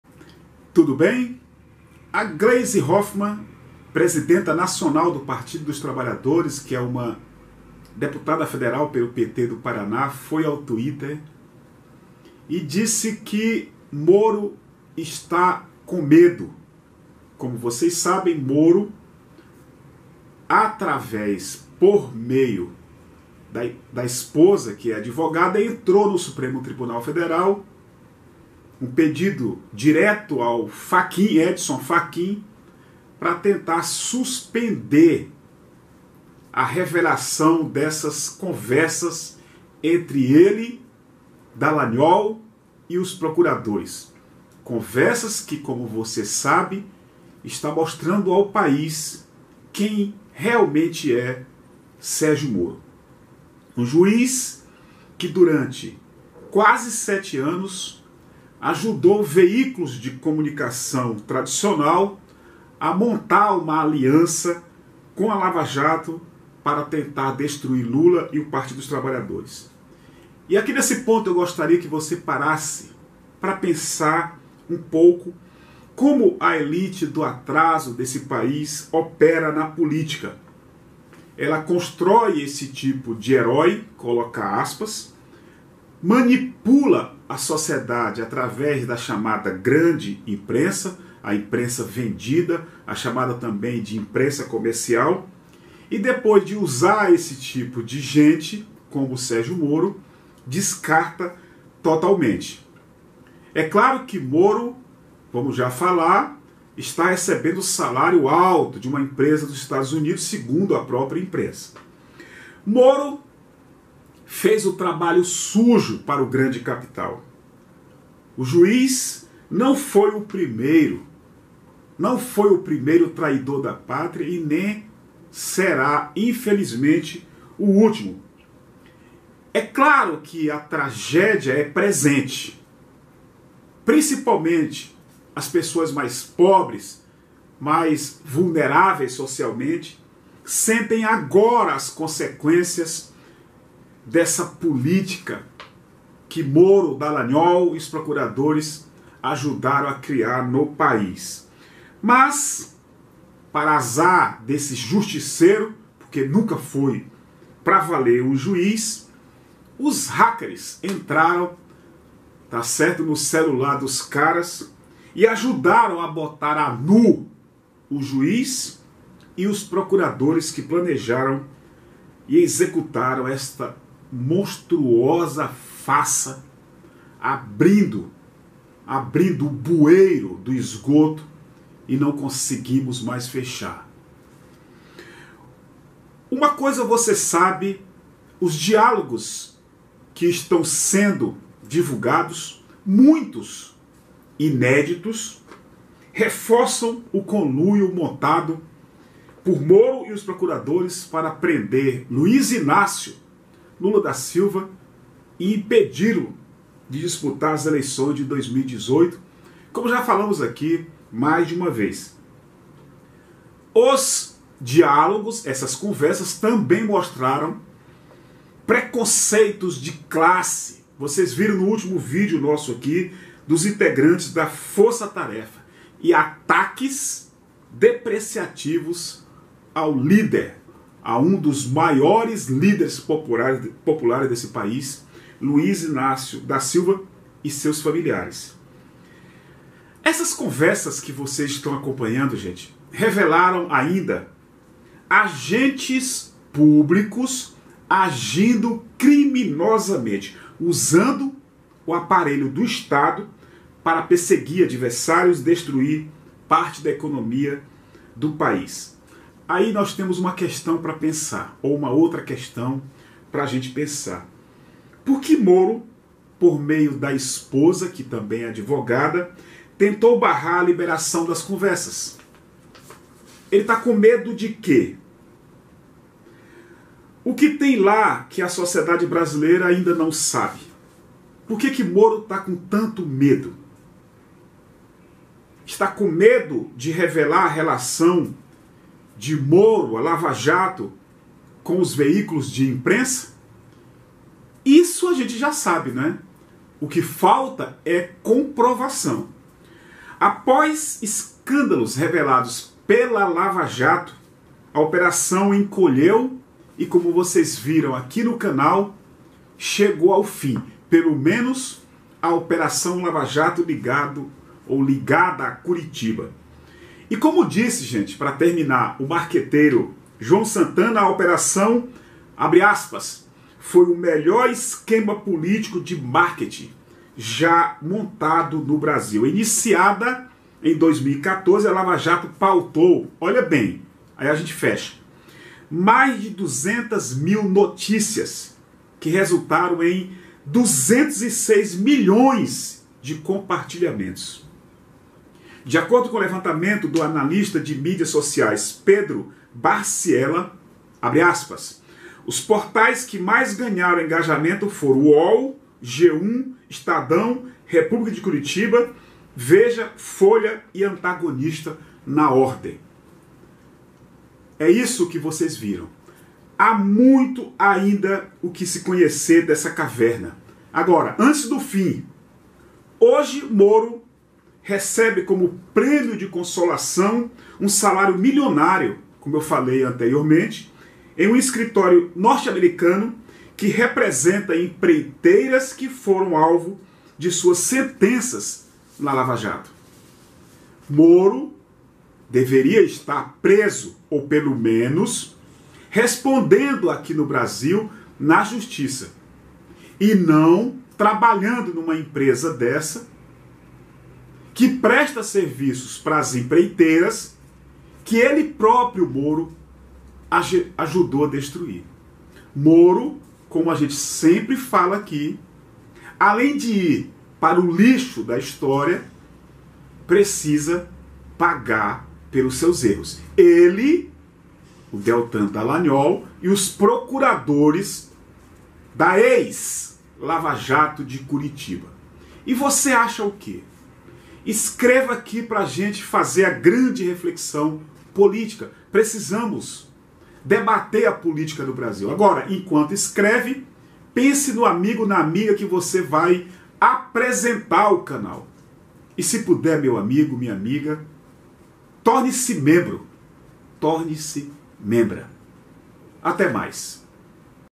MORO, ACUADO E COM MEDO DA PRISÃO (5/2/2021)
“Moro entra no STF para evitar liberação das conversas que provam seu conluio contra Lula e diz que agiu para proteger o ex-presidente. Será que ele acha que alguém vai cair nesse papo furado? Faz-me rir! Tá é com medo de tudo o que está vindo à tona!”, disse Gleisi.
(Foto: Valter Campanato/Agência Brasil - Stuckert - Divulgação)

APOIE O CANAL AQUI:
CONTA PARA CONTRIBUIÇÃO:
Caixa Econômica Federal - Agência: 3314 - Operação: 013 - Conta Poupança: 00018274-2